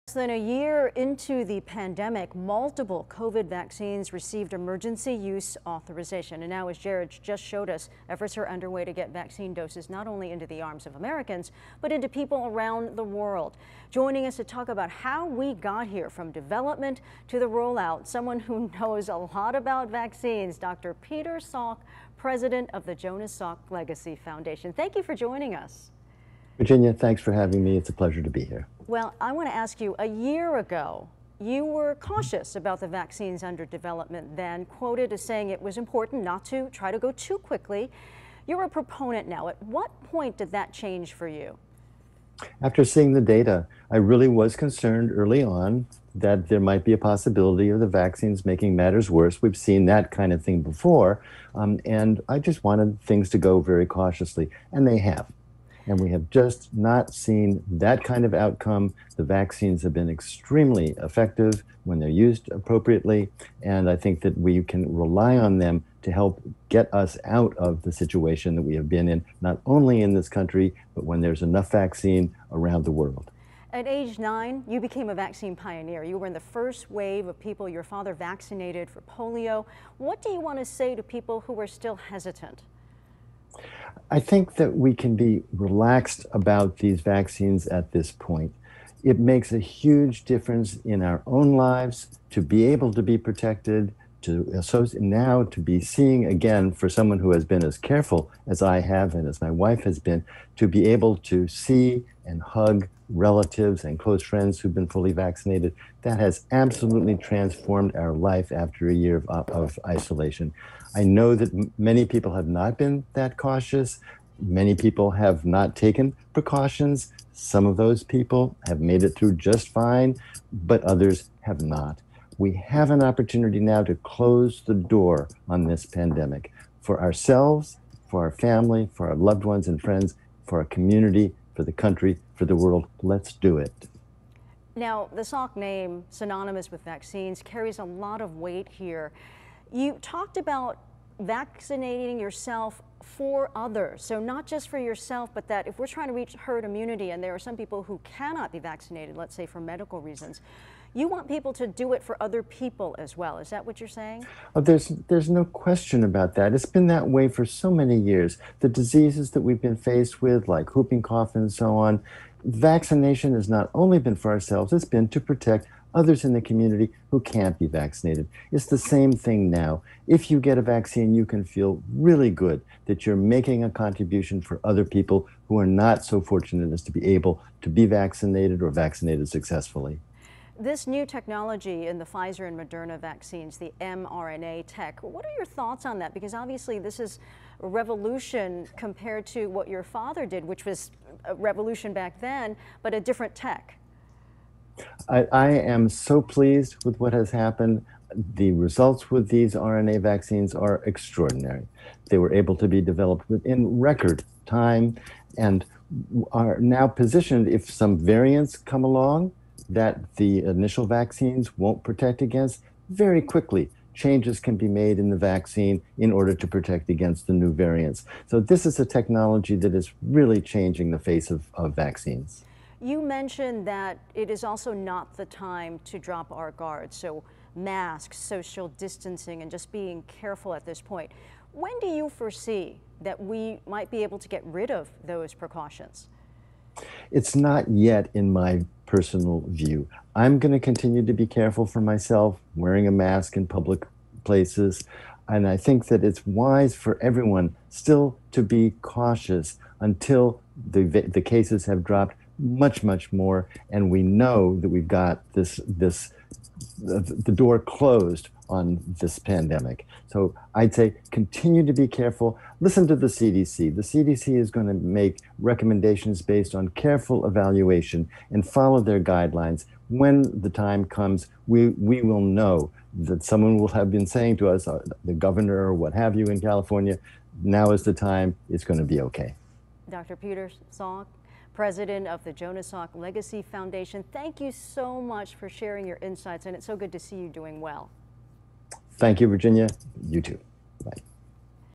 Salk on COVID vaccine development
Dr. Peter Salk, the president of the Salk Legacy Foundation, talks to ABC 10News about the development of the COVID-19 vaccine.